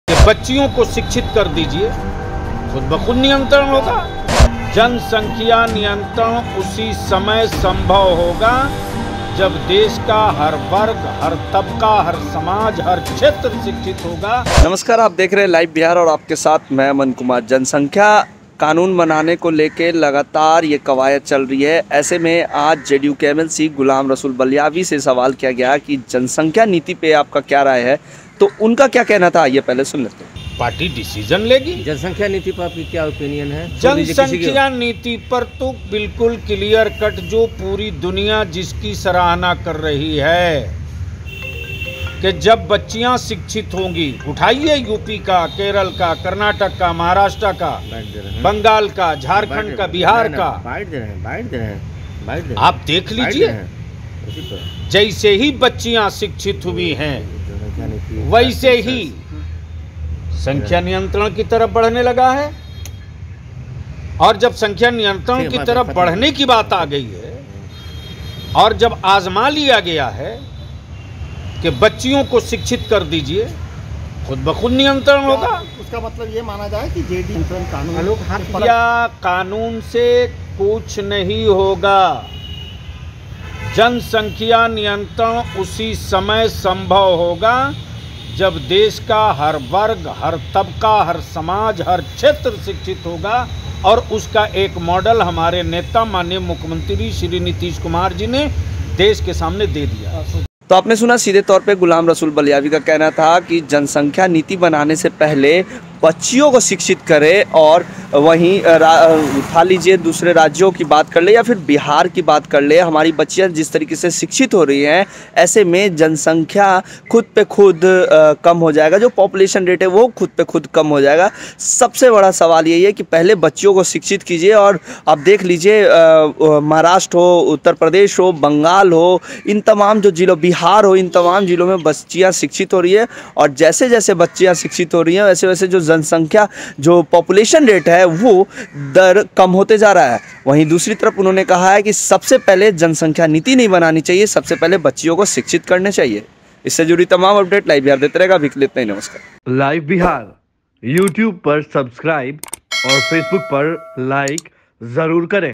JDU एमएलसी Gulam Rasul Biliyawi ने जनसंख्या कानून बनाने को लेकर क्या कहा सुनिए | Live Bihar
जेडीयू एमएलसी गुलाम रसूल बलियावी ने जनसंख्या कानून बनाने को लेकर क्या कहा सुनिए